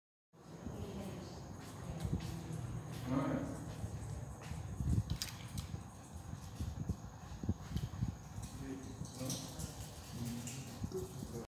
[Free Video Footage] LOOP FLOWER IN THE WIND
FreeFootage video of artificial flowers in the breeze wind.